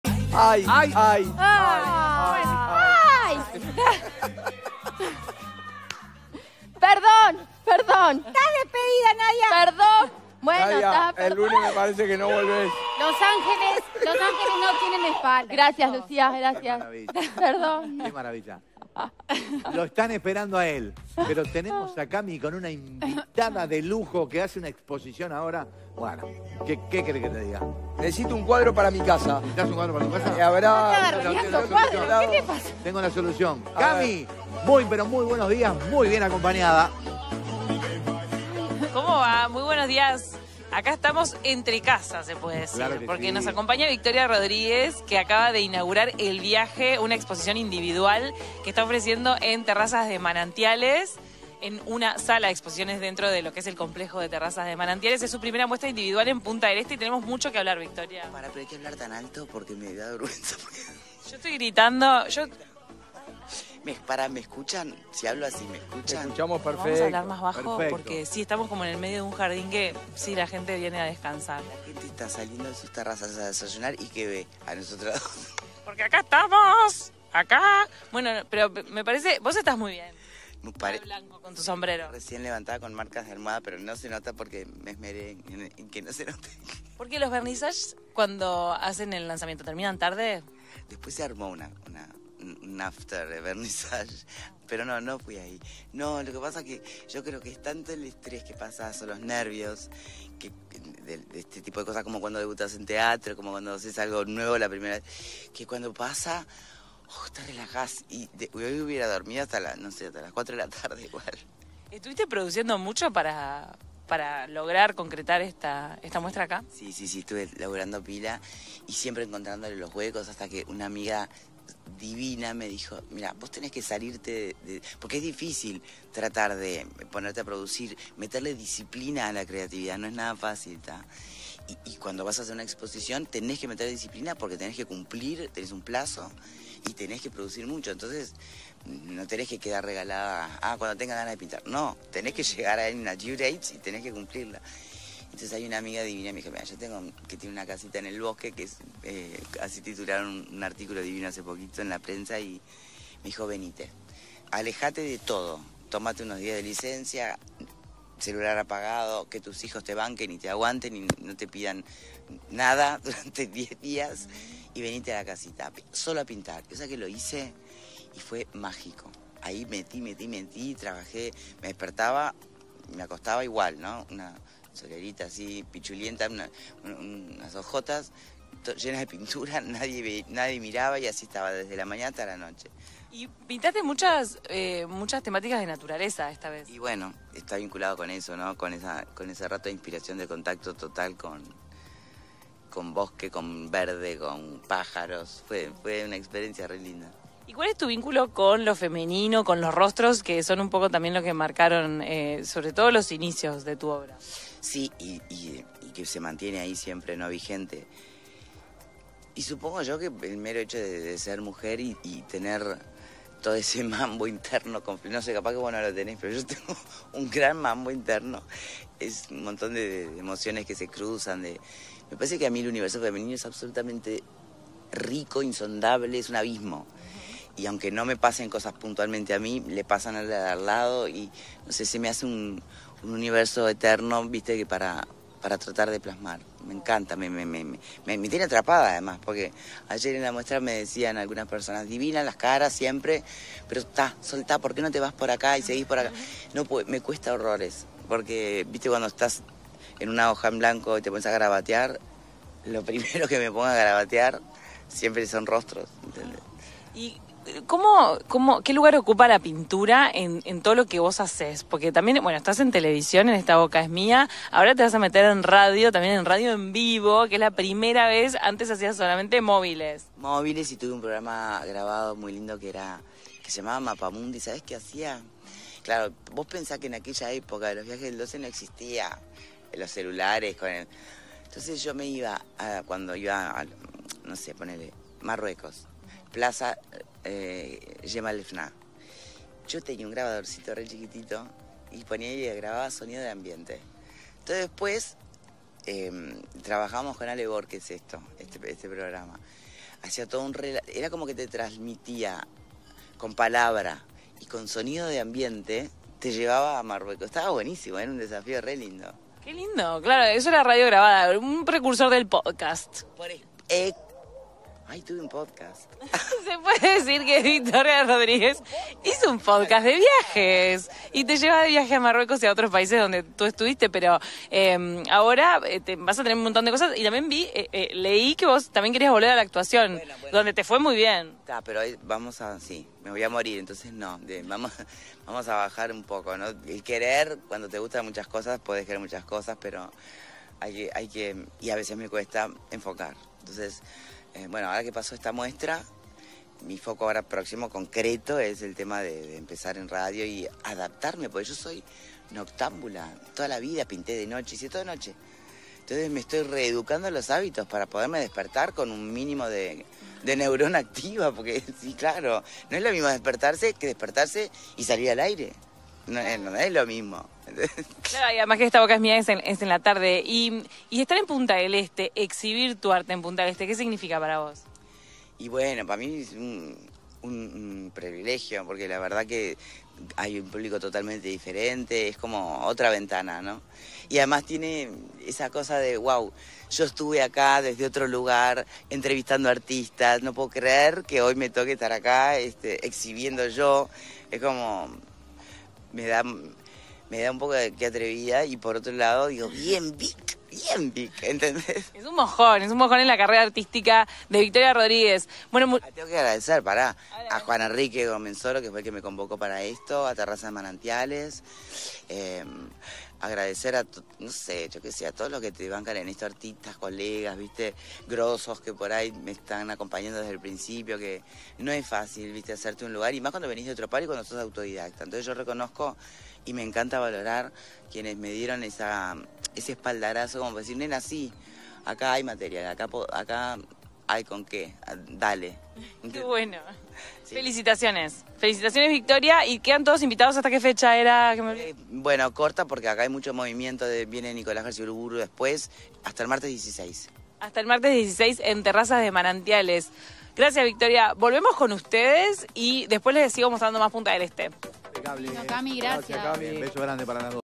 Victoria Rodríguez presenta su nueva obra: "El viaje"
Hablamos con la comunicadora y artista, Victoria Rodríguez.